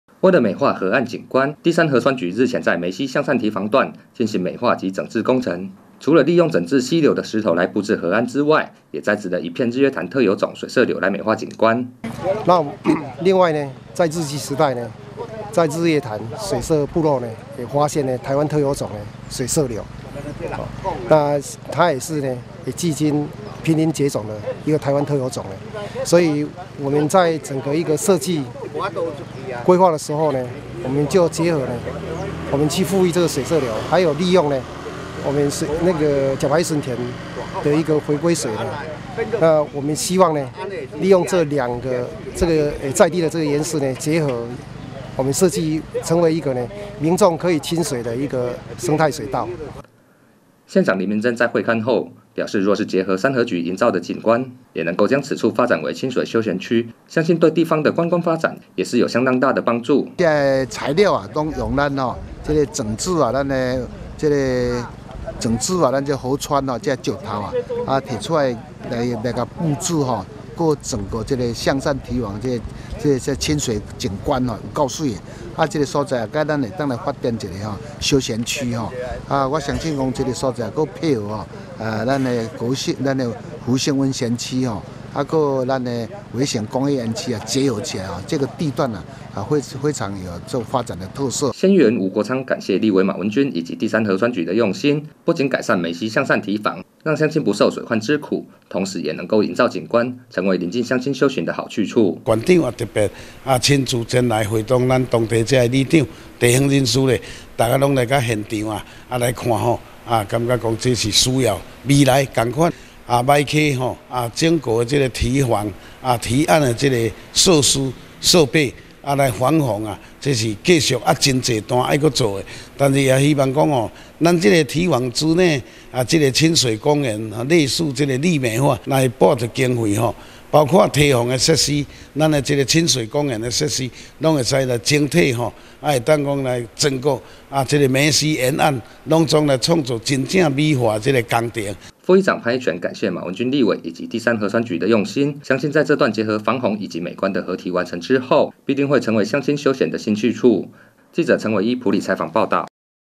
20170105 民議新聞 眉溪向善堤防景觀美化 結合鄰近區域打造新景點(縣議員 吳國昌)
埔里鎮眉溪向善堤防日前在馬文君立委爭取下，建設了一個集合美觀以及實用性的堤防，副議長潘一全感謝馬立委以及三河局的用心，相信在堤防完工之後也能夠成為鄰近鄉親休閒的新景點。